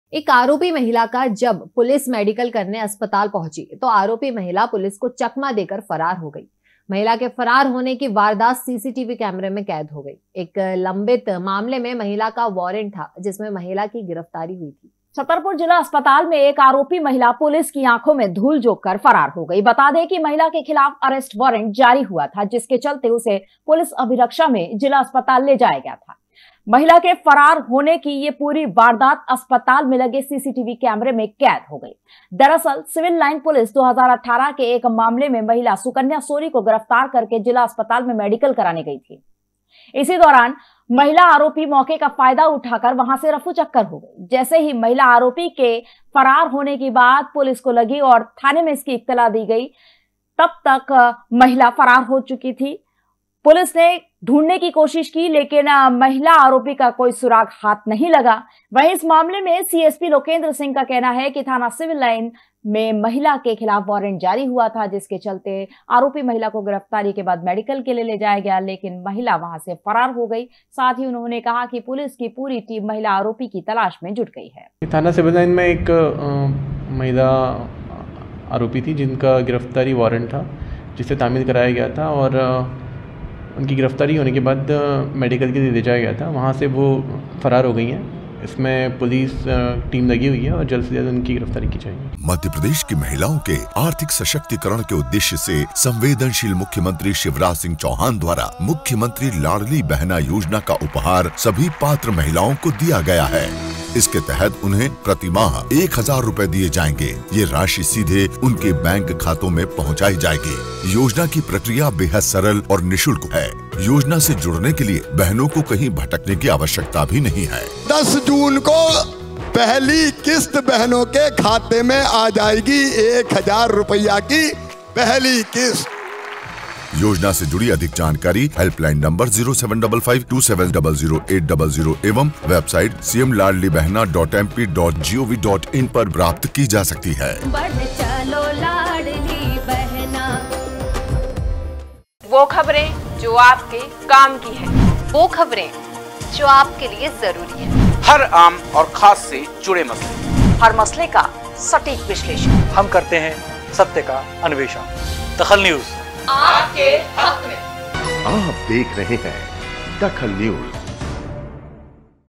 DAKHAL NEWS पुलिस को चकमा देकर फरार महिला आरोपी
पुलिस को चकमा देकर फरार महिला आरोपी
महिला अस्पताल में ईलाज के दौरान हुई फरार

news
daily news
madhyapradesh news
dakhal news
today news
crime news
news updates
viral
trending